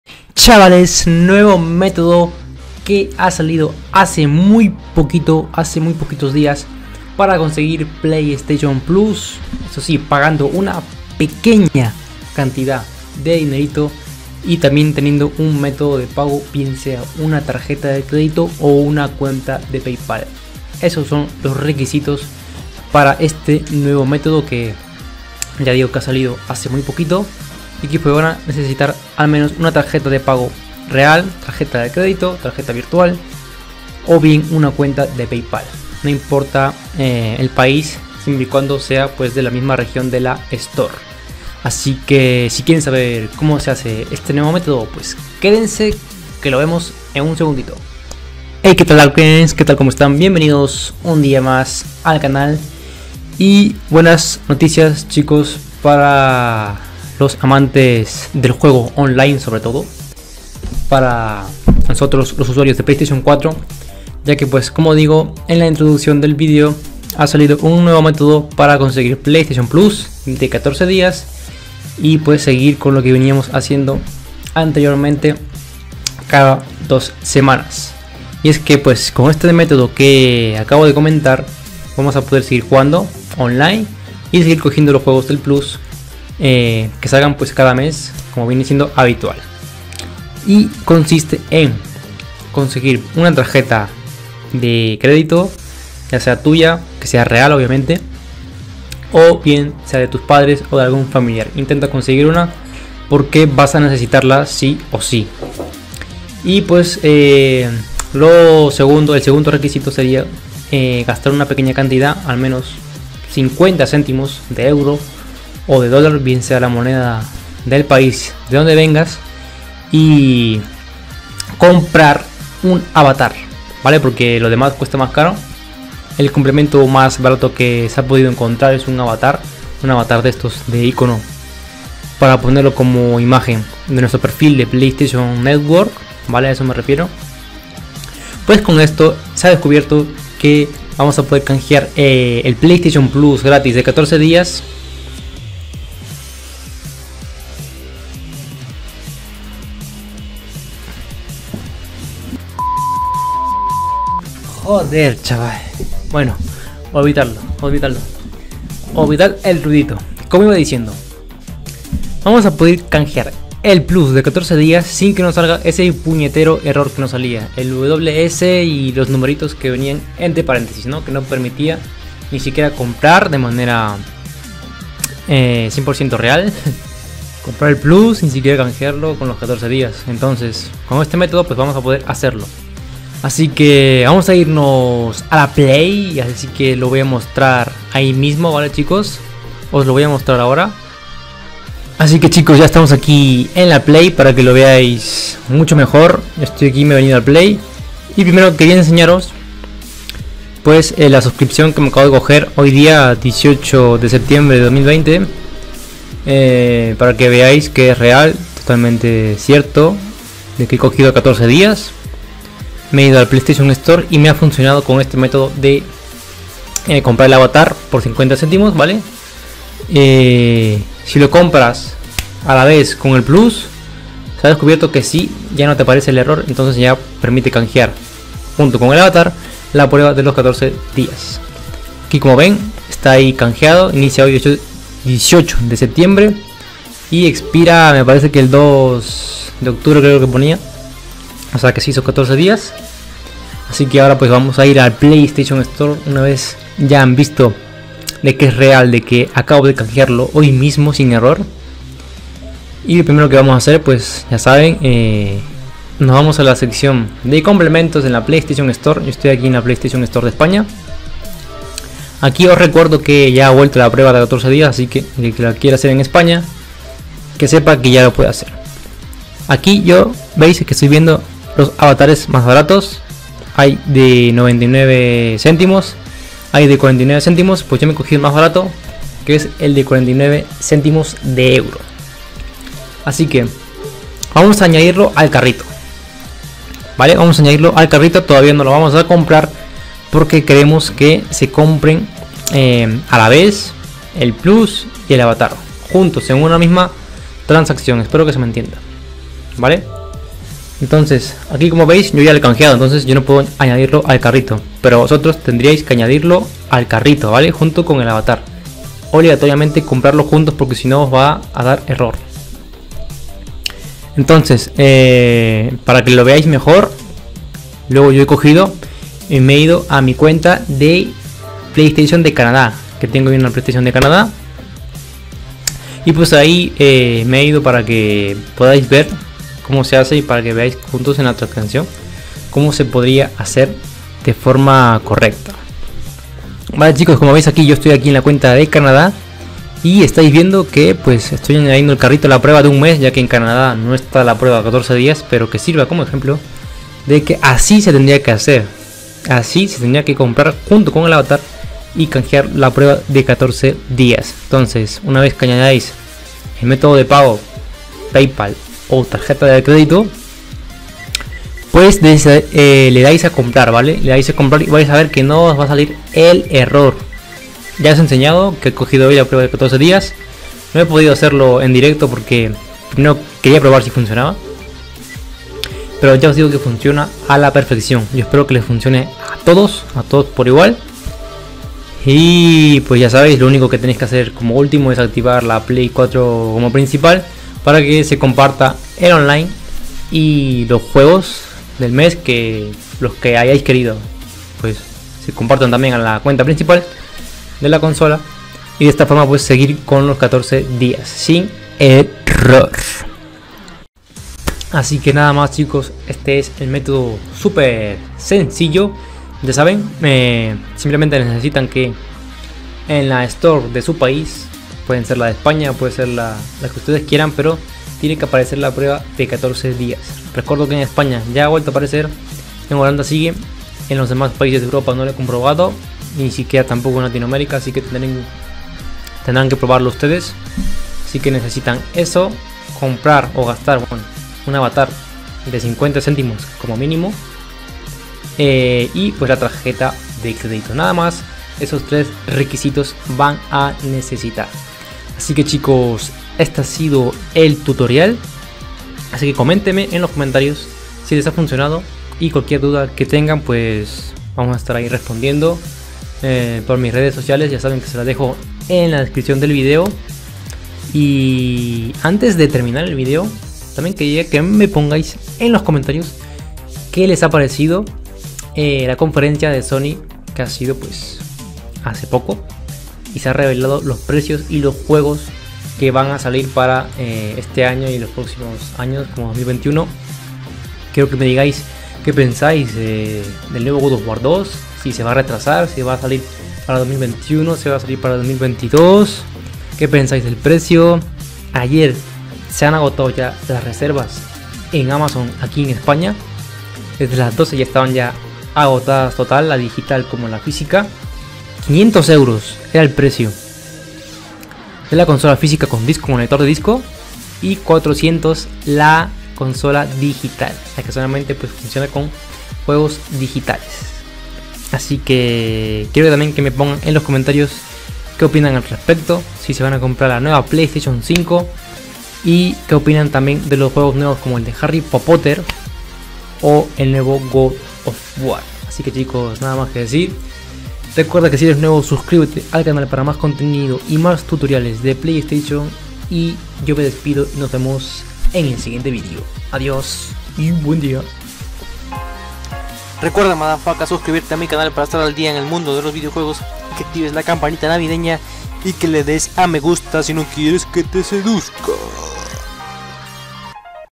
🔥 RÁPIDO🔥* NUEVO* METODO PS PLUS OCTUBRE 2020 SOLUCION ERROR WS
SOLUCION DEFINITIVA AL ERROR DE PS PLUS WS - 43689-0
CONSIGUE TU PRUEBA DE 14 DIAS DE PS PLUS CON ESTE NUEVO METODO!!
PS PLUS NUEVO METODO OCTUBRE 2020

Recuerda seguir al pie de la letra las instrucciones del video. *Avatar y los 14 dias de ps plus deben comprarse juntos*. (AÑADELOS AL CARRITO)

⚠️NO VENGAS CON LA IDEA DE QUE TE VOY A DAR BINS, EN MI CANAL NO UTILIZO MÉTODOS ILEGALES.⚠️